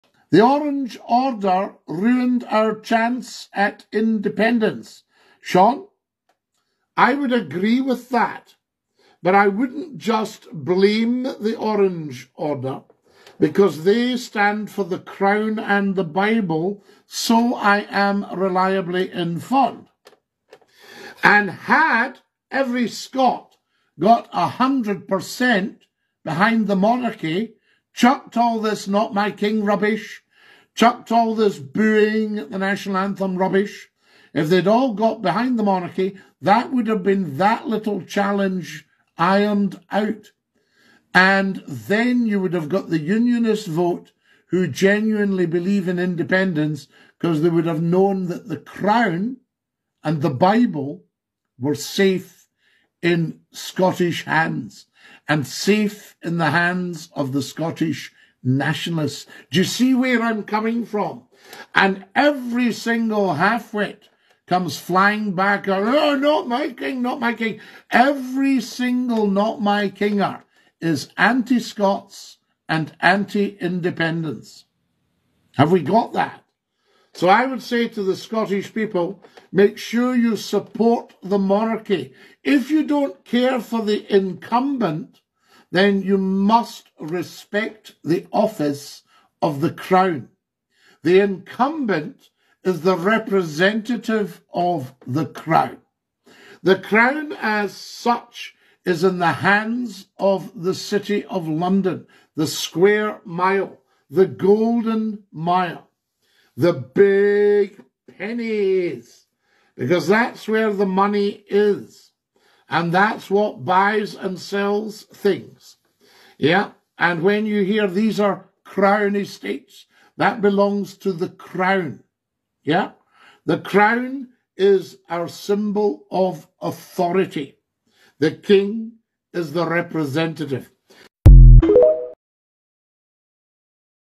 SHOULD SCOTLAND EXPEL ANTI-MONARCHISTS 🤔
SHOULD SCOTLAND EXPEL ANTI-MONARCHISTS BEFORE INDYREF 2🤔
LEGENDARY BROADCASTER AND INTERNET ICON SCOTTIE MCCLUE ASKS SHOULD SCOTLAND GET 💯 BEHIND THE MONARCHY TO HAVE A CHANCE OF SECURING THE UNIONIST VOTE FOR INDEPENDENCE BEFORE INDYREF 2